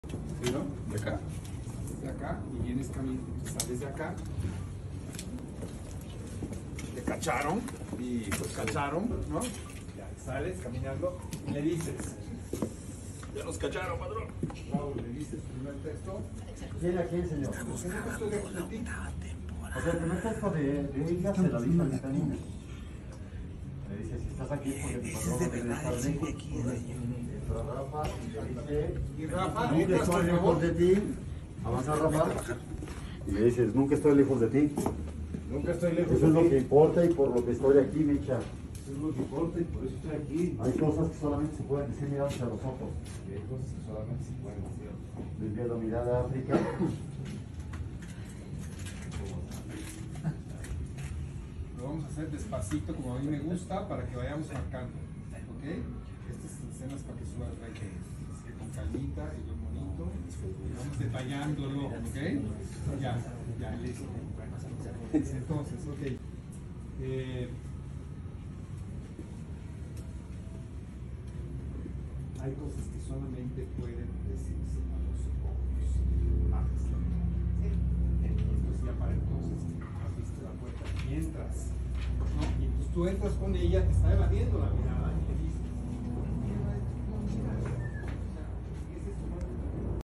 Rafael Amaya en grabaciones de octava temporada | El Diario
Ve el detrás de cámaras del actor Rafael Amaya en las grabaciones de la octava temporada del señor de los cielos.